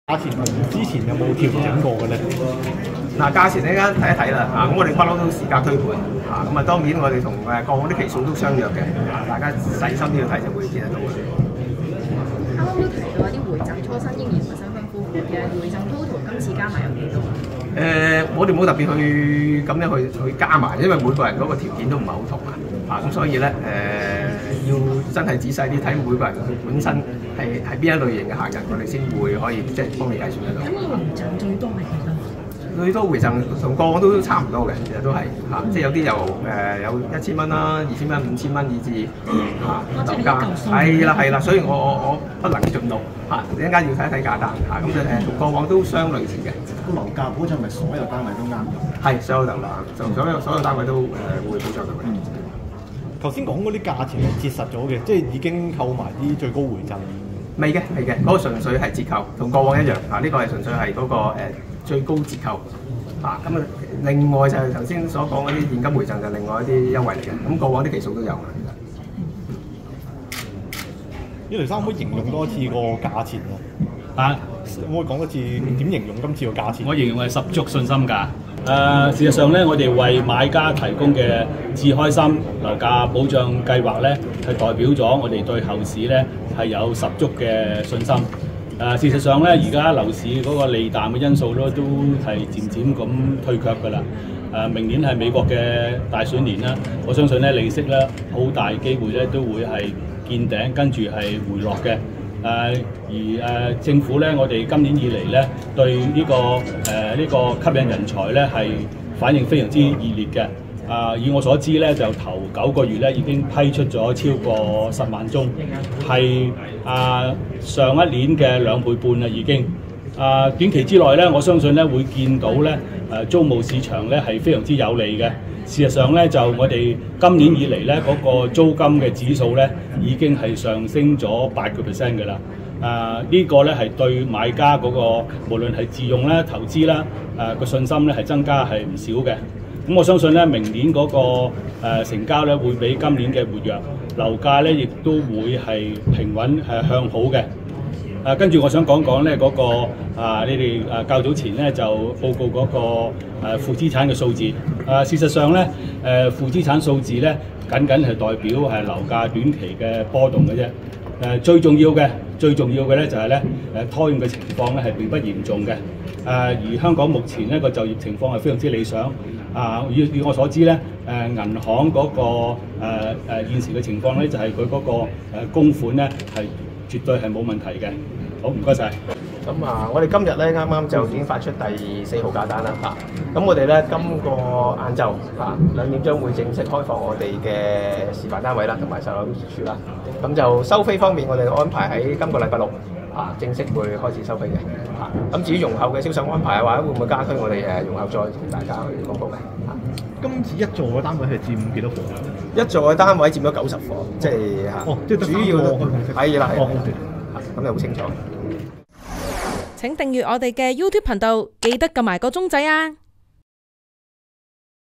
NOVO LAND開價｜第2A期加推93伙 最平335萬 折實均呎13288元重返2月水平｜屯門新盤
本月焦點全新盤、新鴻基地產(016)屯門NOVO LAND第2A期今日(1日)公布第4號價單，涉93伙，維持最高15%折扣優惠後，折實售價由335.49萬元，折實平均呎價13,288元，重返項目第2B期今年2月推出首張價單146伙折實平均呎價13,288元。

新地副董事總經理雷霆以「十足信心價」形容今批單位定價，強調一向以市價推盤，是次提供3種付款方法，另提供「置稱心」按揭利息保障及全新「置開心」樓價保障，「置開心」樓價保障金額上限為樓價5%，保障期至明年5月份。